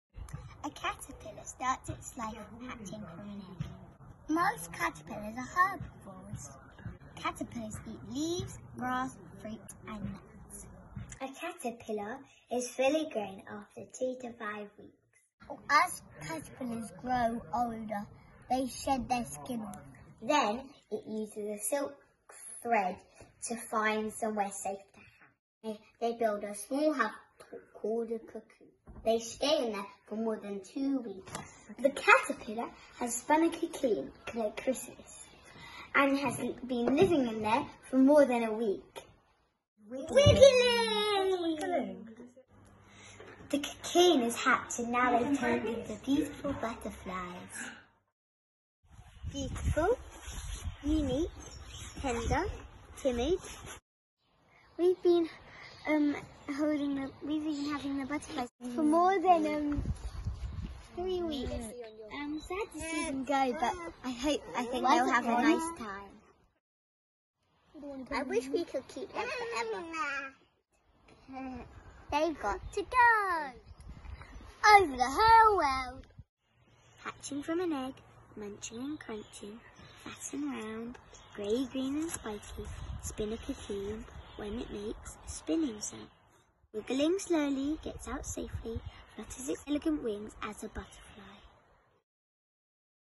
As a Butterfly Documentary
Clara and Ira - Age 5 & 7

"A delightful film. I loved the storytelling from caterpillar to butterfly" - Judge, David Lindo (The Urban Birder)